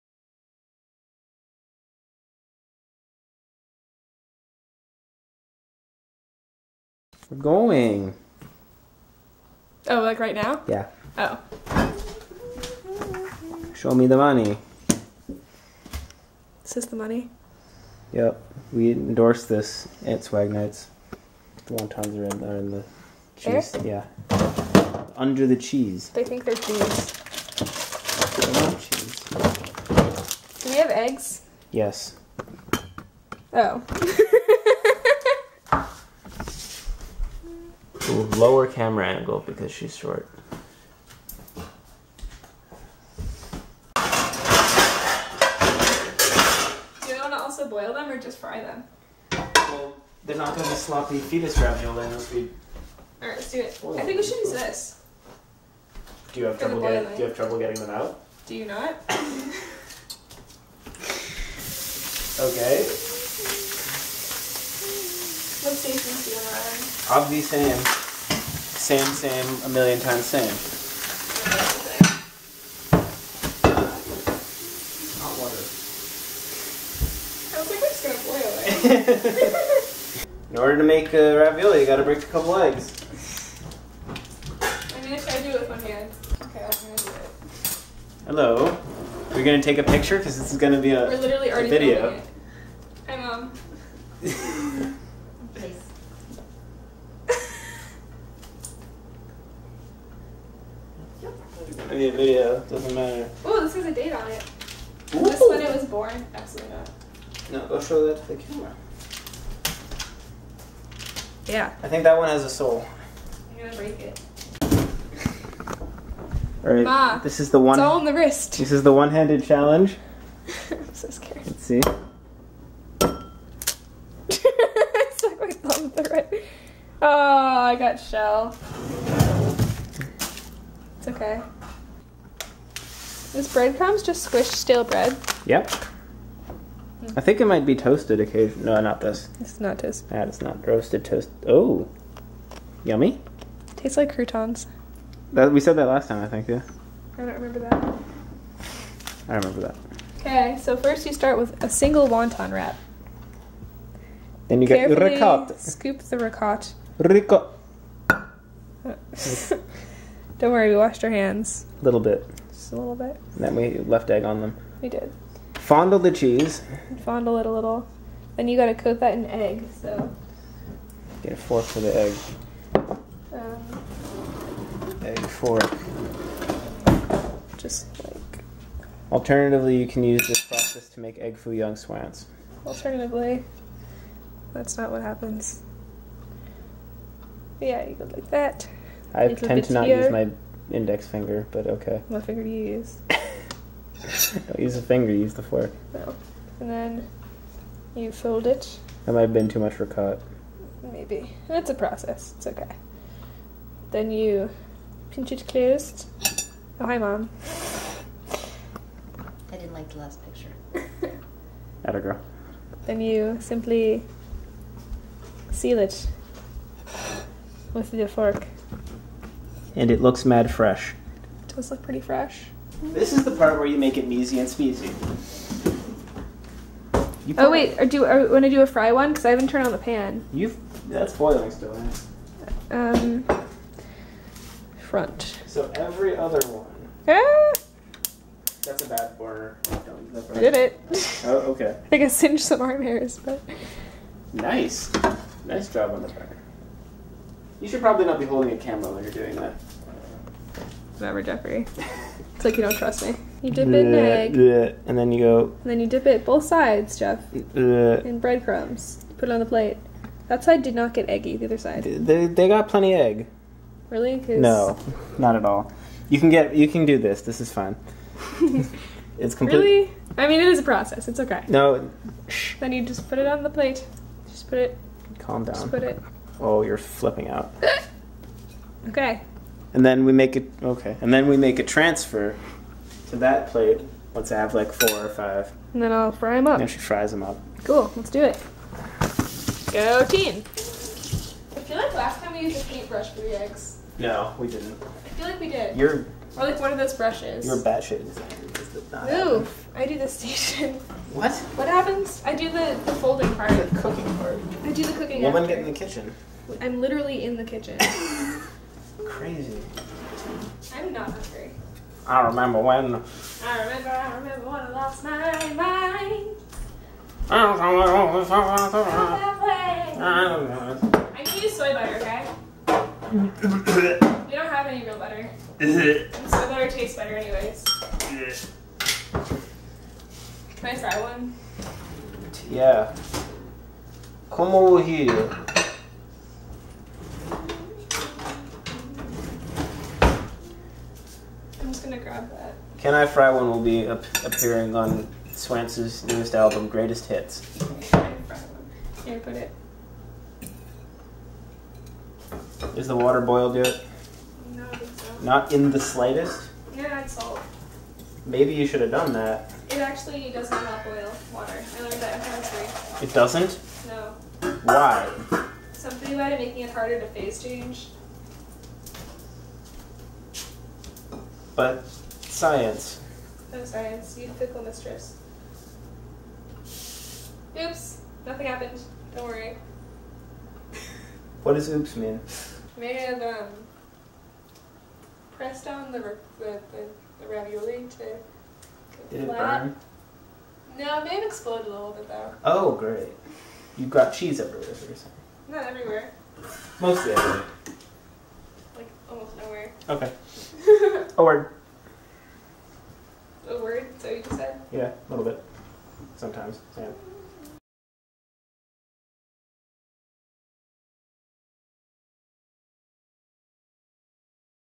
(Salvaged footage) Cooking for Dinguses
Corinne and Jeffrey make ravioli. This is not Italian Cooking 2, nor was it ever trying to be. My camera really needs to work on its anger issues though.